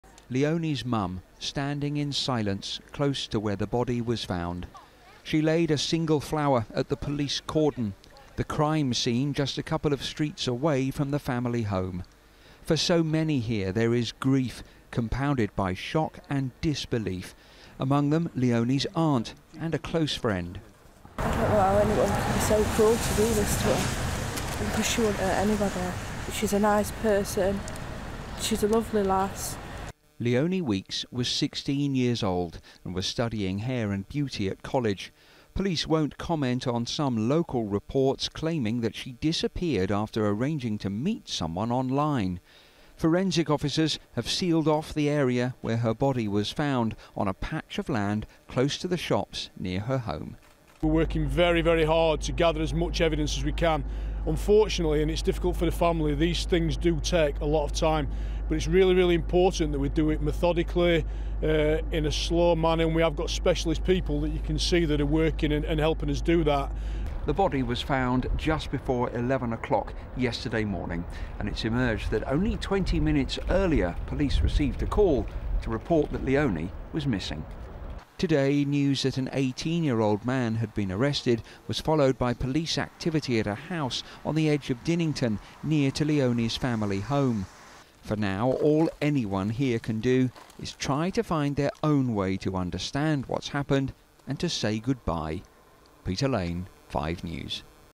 Sixteen-yearold girl found dead in an alleyway
An 18-year-old man is being questioned on suspicion of murdering a girl, whose body was found on a pathway in South Yorkshire. The girl, named as 16-year-old Leonne Weeks, had been reported missing in Rotherham yesterday morning when she failed to return home.